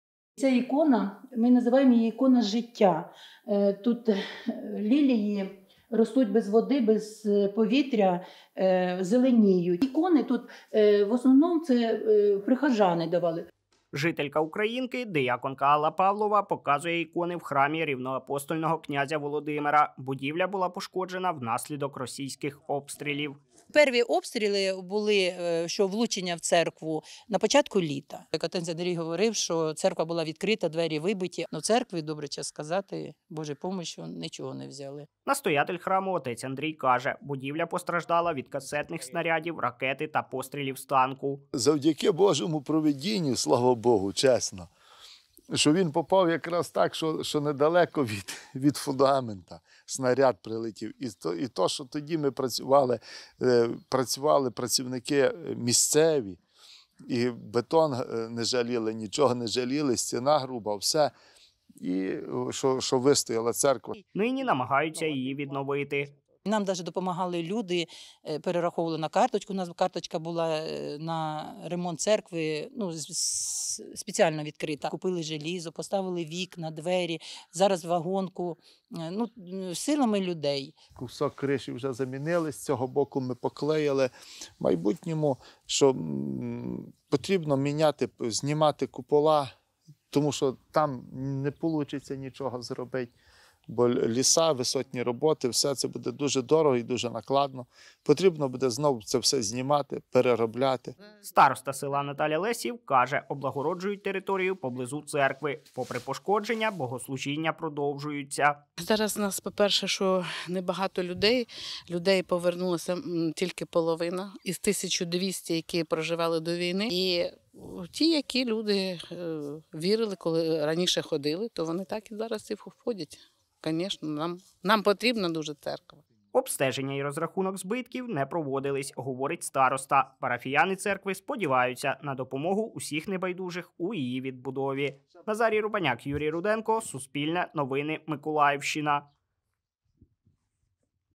Обстрілювали ракетами та з танків. На Миколаївщині відновлюють церкву
На Миколаївщині у селі Українка власними силами віряни відновлюють храм рівноапостольного князя Володимира. Релігійна споруда була пошкоджена влітку 2022 року.